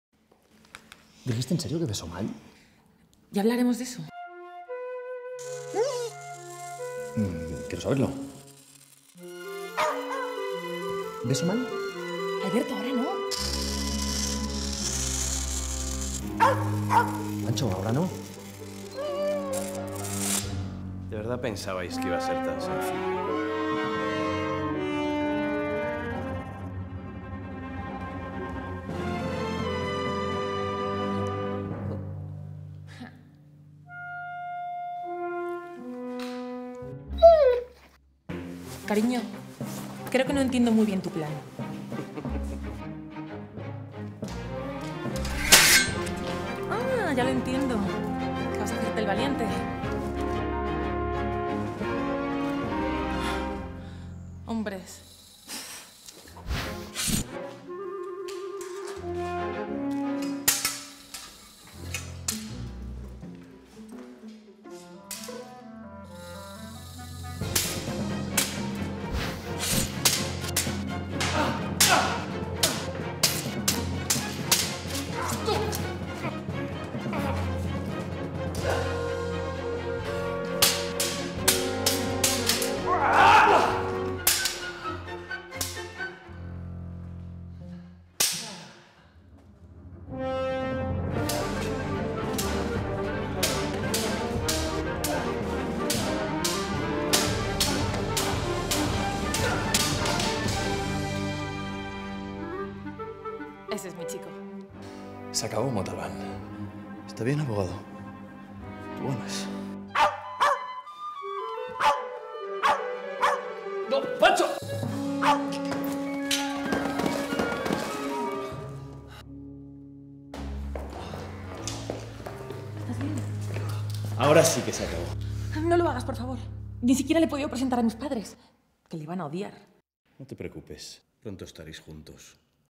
Comedic Sword Fight
This is a rescore that I composed in 2018 for a scene from a Spanish family film.